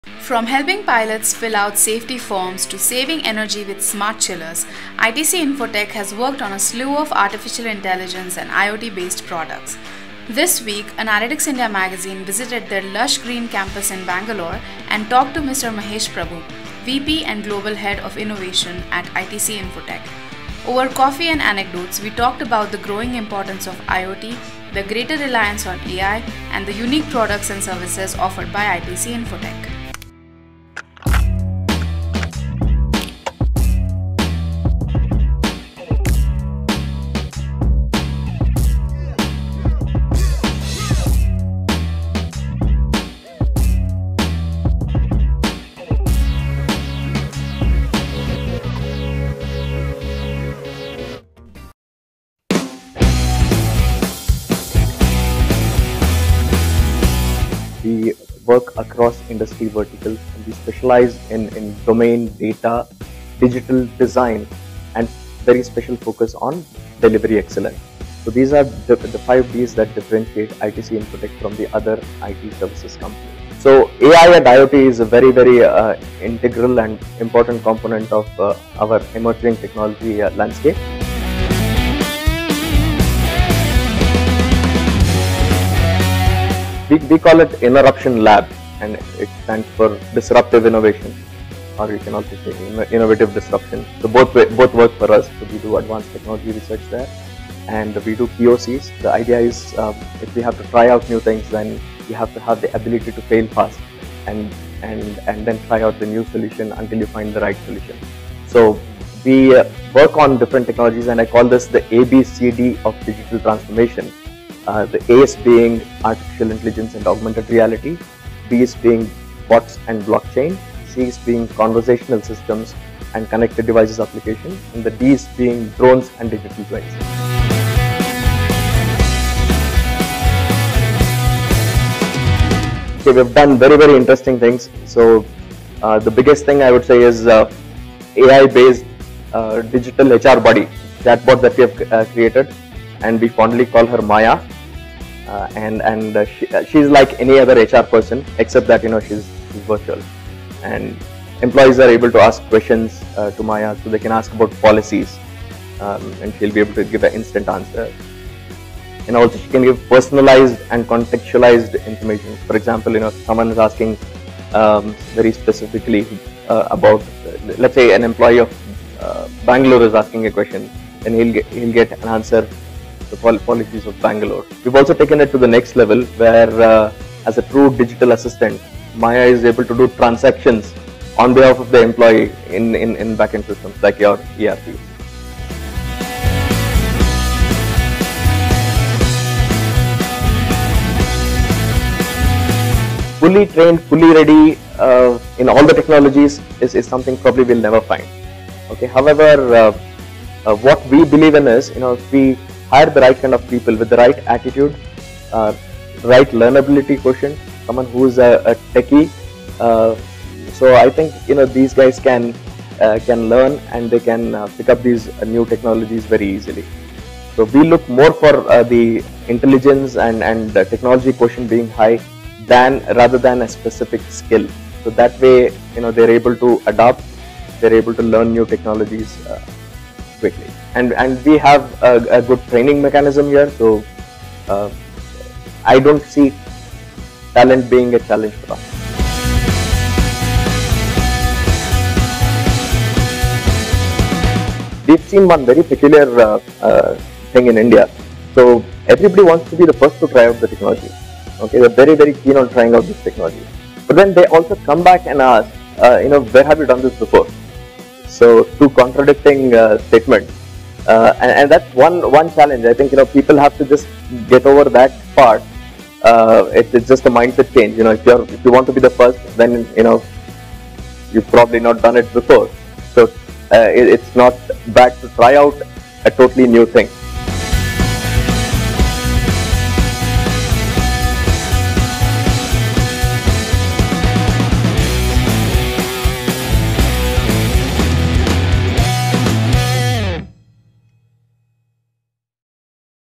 Deeper Insights: With Mahesh Prabhu, VP and Global Head of Innovation at ITC Infotech
Analytics India Magazine got in touch with Mahesh Prabhu, a technology enthusiast and evangelist, who has been instrumental in the incubation of cutting-edge technologies, research and innovation, and broad-basing the capability enhancement programs.